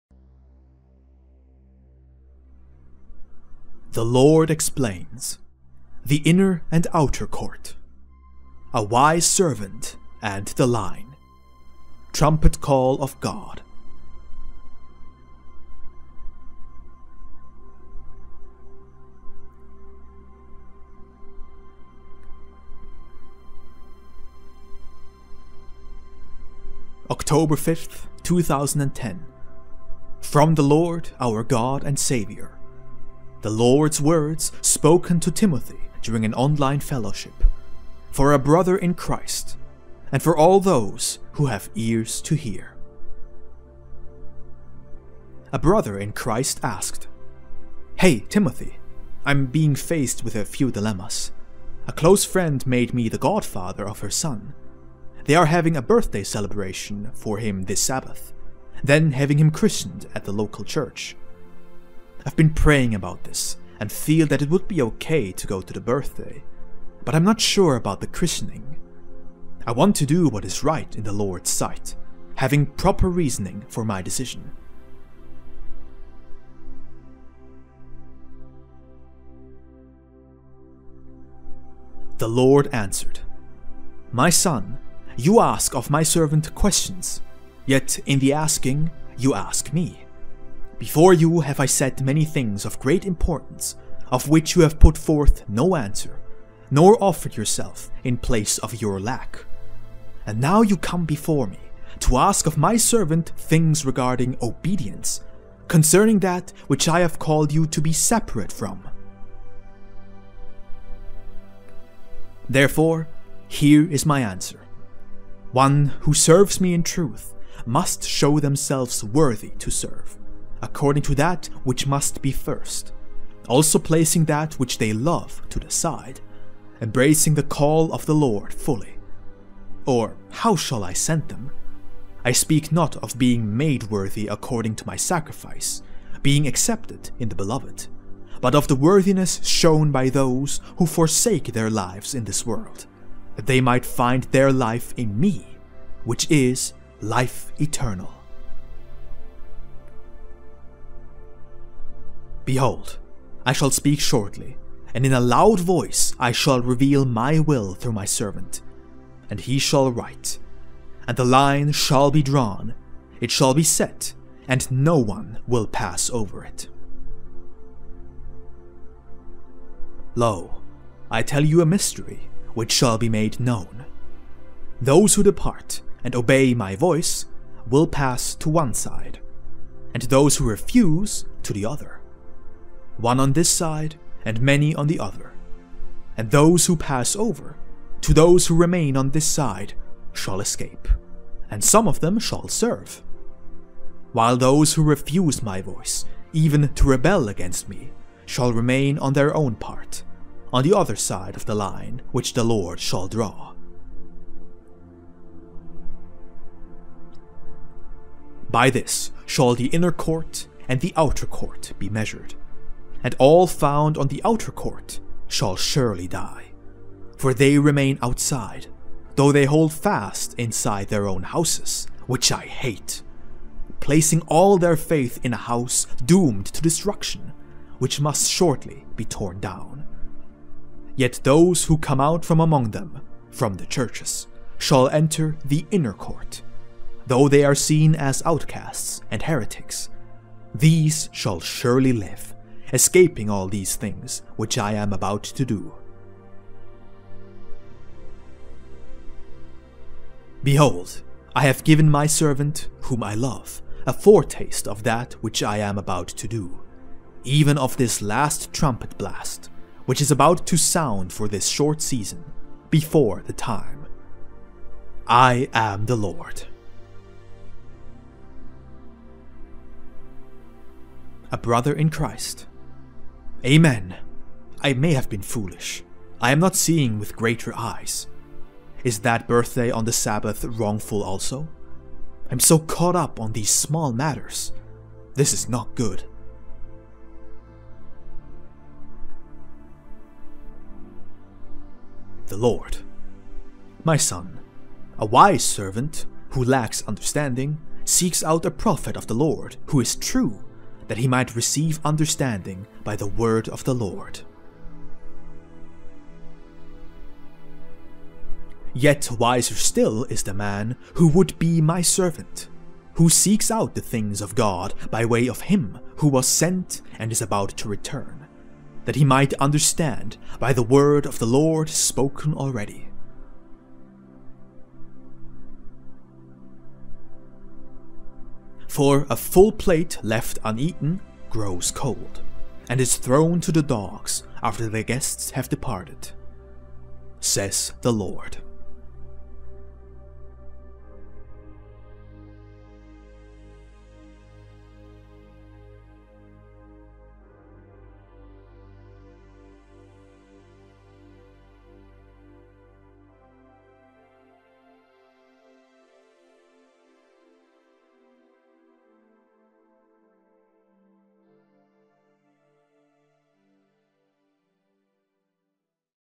The inner and outer Court & A wise Servant and the Line 🎺 Trumpet Call of God
The inner and outer Court & A wise Servant and the Line

October 5, 2010 – From The Lord, Our God and Savior – The Lord’s Words spoken to Timothy during an Online Fellowship – For a Brother in Christ, and for all Those who have Ears to hear

A Brother in Christ asked… Hey Timothy, I’m being faced with a few dilemmas… A close friend made me the godfather of her son. They are having a birthday celebration for him this Sabbath, then having him christened at a local church. I’ve been praying about this, and feel that it would be okay to go to the birthday, but I’m not sure about the christening. I want to do what is right in The Lord’s sight, having proper reasoning for my decision.

[The Lord answered] My Son, you ask of My servant questions, yet in the asking you ask Me… Before you have I set many things of great importance, of which you have put forth no answer, nor offered yourself in place of your lack. And now you come before Me, to ask of My servant things regarding obedience, concerning that which I have called you to be separate from…

Therefore, here is My answer… One, who serves Me in truth, must show themselves worthy to serve, according to that which must be first, also placing that which they love to the side, embracing the call of The Lord fully… Or how shall I send them? I speak not of being made worthy according to My sacrifice, being accepted in The Beloved, but of the worthiness shown by those who forsake their lives in this world, that they might find their life in Me… Which is life eternal.

Behold, I shall speak shortly, and in a loud voice I shall reveal My will through My servant, and he shall write… And the line shall be drawn, it shall be set, and no one will pass over it.

Lo, I tell you a mystery, which shall be made known… Those who depart, and obey My voice, will pass to one side; and those who refuse to the other… One on this side, and many on the other. And those who pass over, to those who remain on this side, shall escape, and some of them shall serve. While those who refuse My voice, even to rebel against Me, shall remain on their own part, on the other side of the line which The Lord shall draw…

By this shall the inner court and the outer court be measured. And all found on the outer court shall surely die, for they remain outside, though they hold fast inside their own houses, which I hate… Placing all their faith in a house doomed to destruction, which must shortly be torn down. Yet those, who come out from among them (from the Churches), shall enter the inner court, though they are seen as outcasts and heretics… These shall surely live, escaping all these things which I am about to do.

Behold, I have given My servant, whom I love, a foretaste of that which I am about to do… Even of this last trumpet blast, which is about to sound for this short season, before the time. I AM THE LORD.

[A Brother in Christ] Amen. I may have been foolish, I am not seeing with greater eyes… Is that birthday on the Sabbath wrongful also? I’m so caught up on these small matters. This is not good.

[The Lord] My son, a wise servant, who lacks understanding, seeks out a prophet of The Lord who is true, that he might receive understanding by the word of The Lord…

Yet wiser still is the man who would be My servant, who seeks out the things of God by way of Him, who was sent and is about to return, that he might understand by the word of The Lord spoken already…

For a full plate left uneaten grows cold, and is thrown to the dogs after the guests have departed, says The Lord.